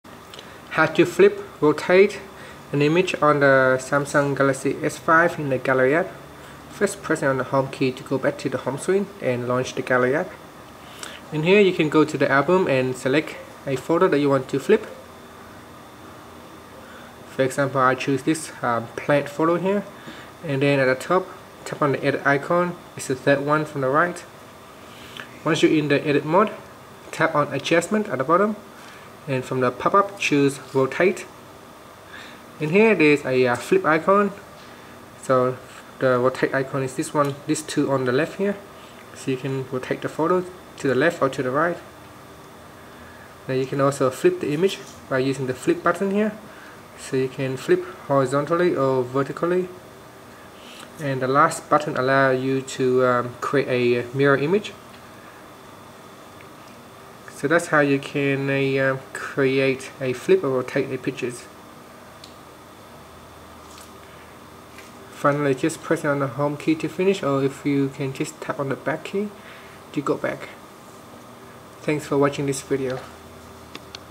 Samsung Galaxy S5: How to Flip / Rotate an Image in Gallery App
Learn how you can flip or rotate an image in the Gallery app on Samsung Galaxy S5.